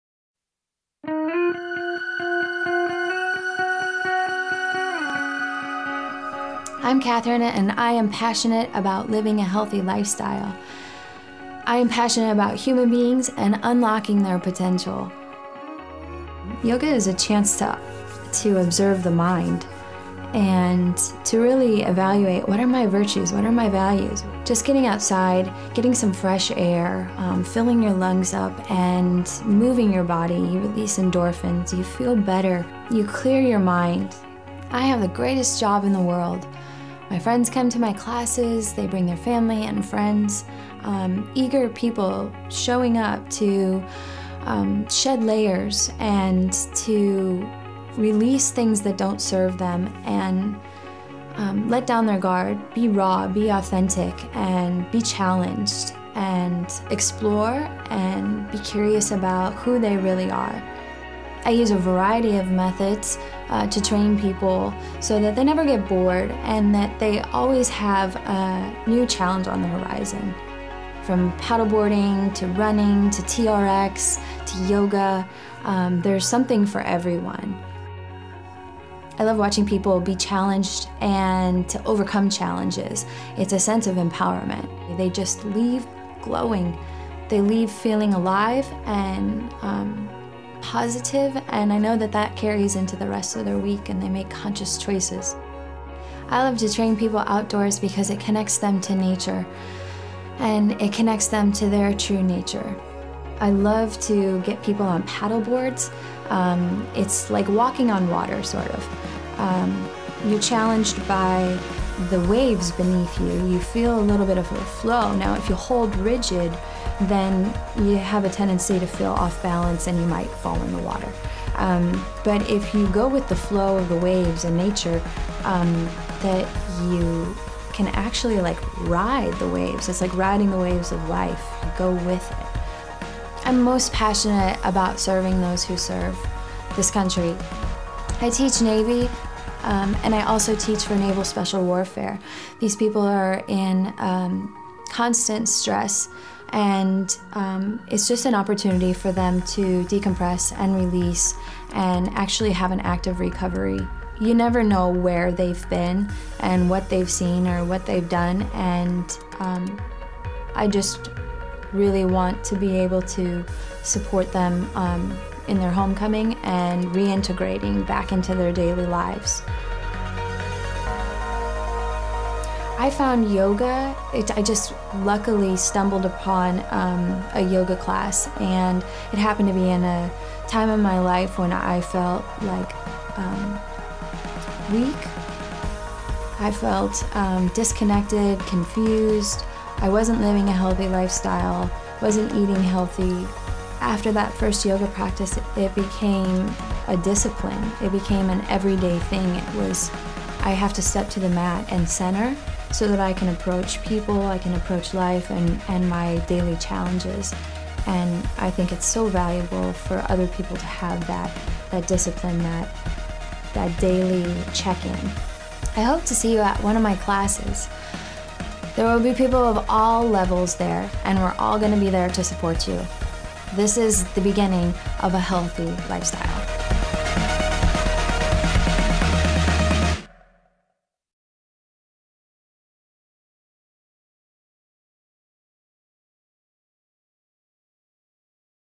Katherine - Fitness Promo Film
We had an incredible time hanging out with Katherine and seeing all the different types of workouts she does with people. She's great at what she does and we hope you'll see it's evident how much she loves what she does in her promo.

Music licensed through With Etiquette